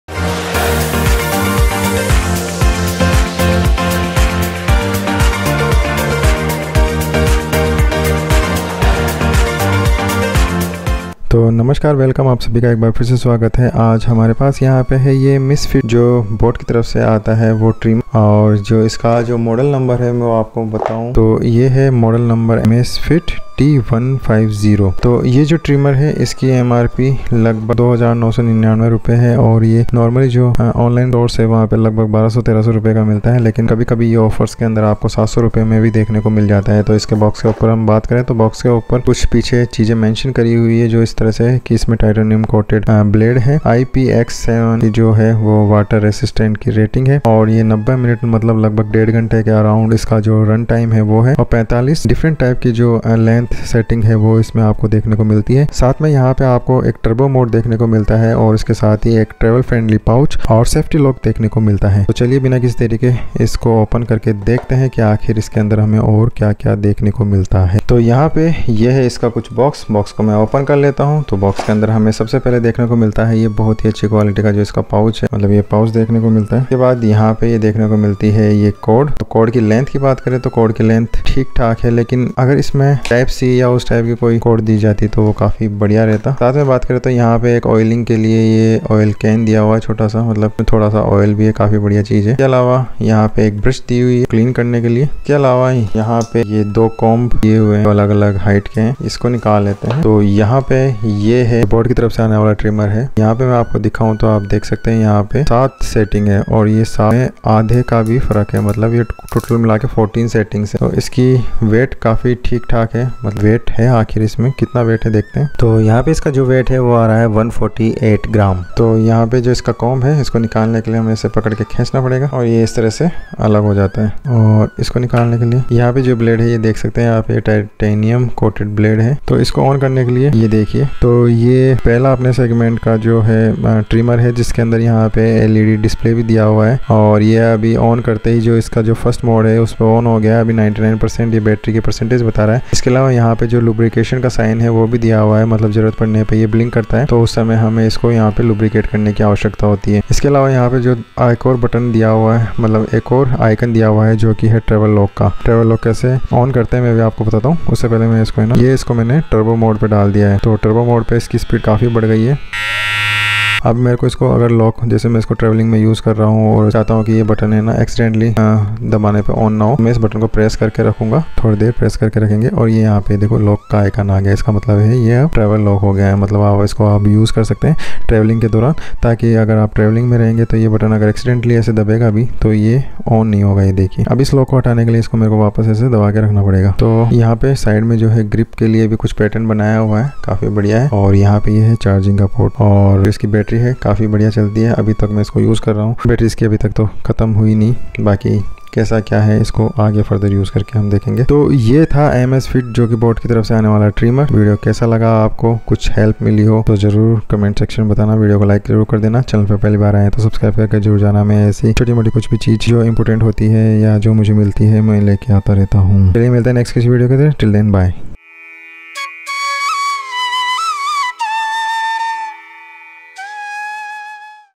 Misfit by boAt T150 Trimmer 90 mins Runtime 45 Length Settings Review in Hindi by itsmkumar
Misfit by boAt T150 Trimmer एक बार चार्ज करने पर चलता है ये 90 मिनट